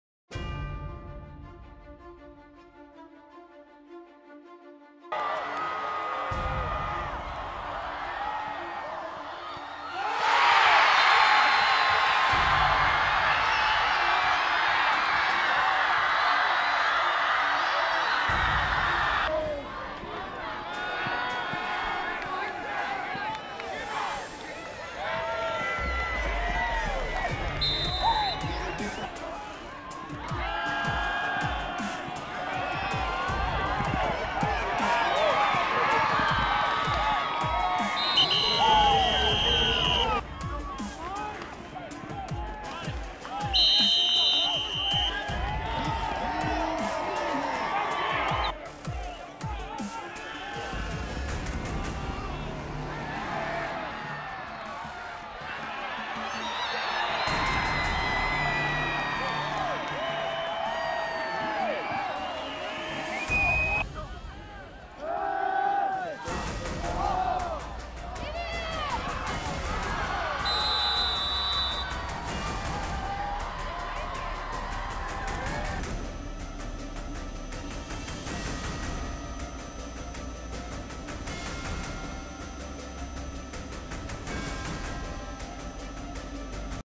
Season highlights: Southridge football 2017
Highlights from Friday Night Flights of the Southridge Skyhawks 2017 football season.